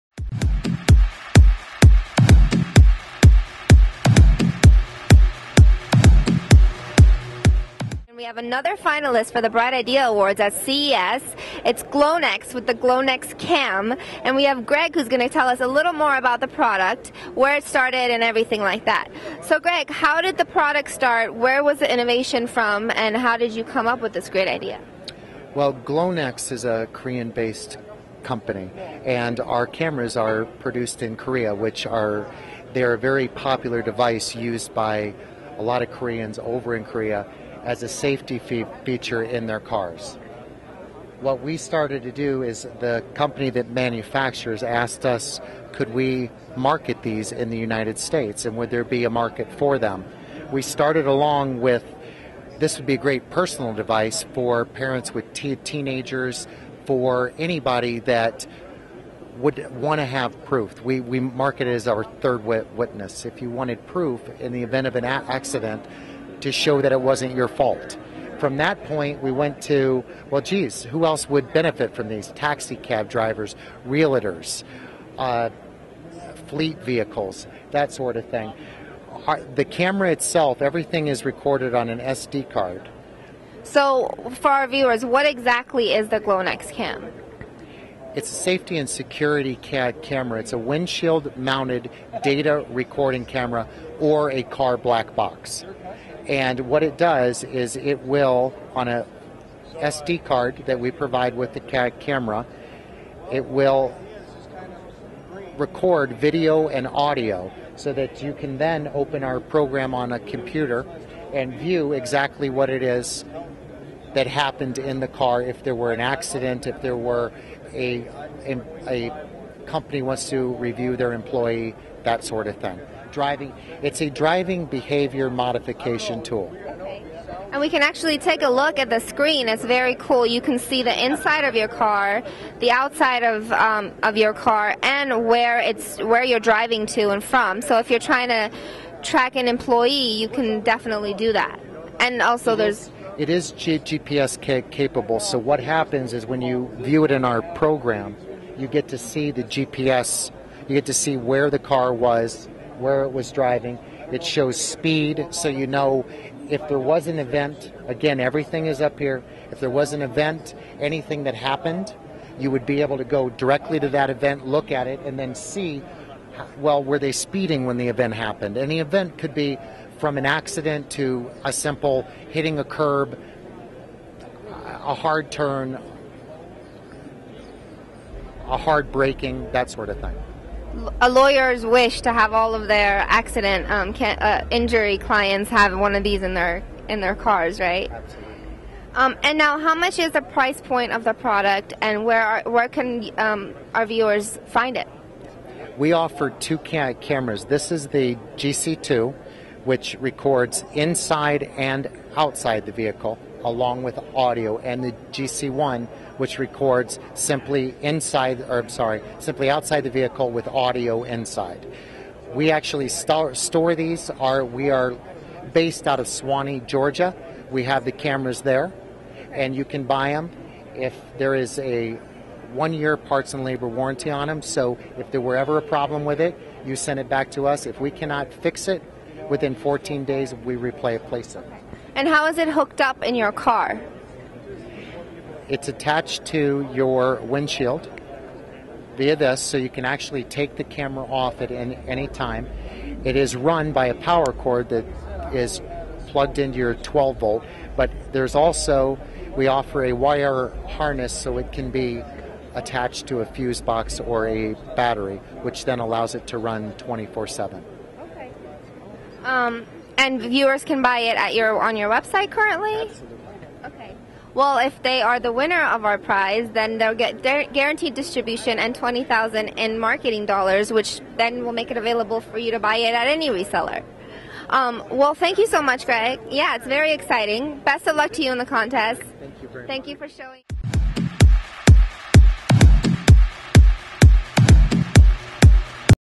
CES 2011- GlobalTechVision interview with Glonex
Glonex showcases its Glocam with indisputable accident evidence technology. Maryam Lahiji of Global Marketing Partners interviews a Glonex representative. Glocam, a windshield audio/video data recorder, provides indisputable accident evidence, is eco-friendly as it increases driver awareness which decreases accidents and vehicle maintenance.